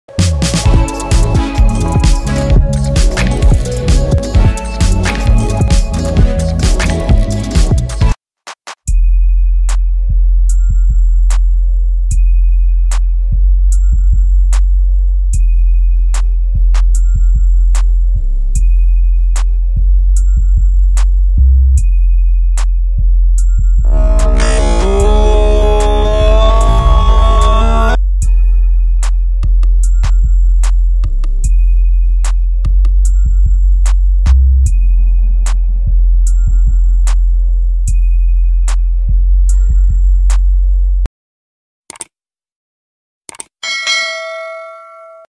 Maheen was crying very badly because of the injury to her hand | Maheen Obaid | Aadii Creation
Hey 👋 Friend I hope this video make you happy 😊, So like the video.